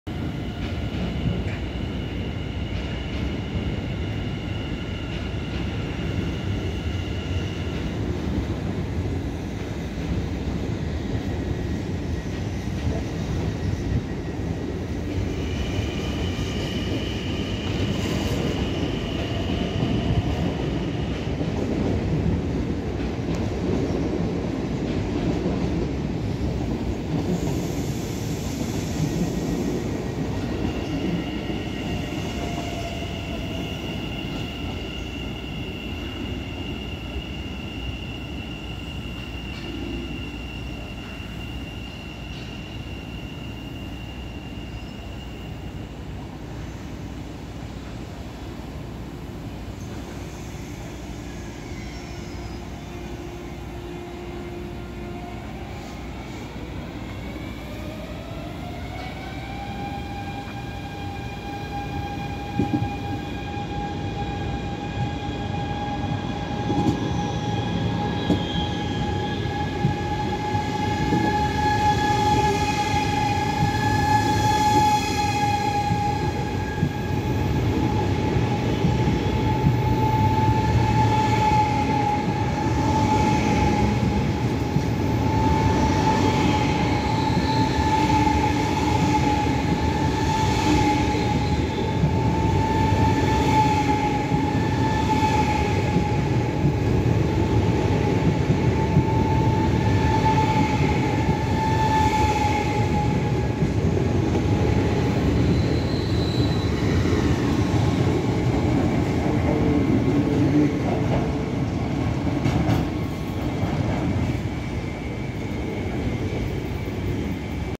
ICE3neo's 8015 en 8018 vertrekken van Amsterdam Centraal
ICE3neo's 8015 en 8018 vertrekken op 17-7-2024 van Amsterdam Centraal als ICE225 naar Köln Messe/Deutz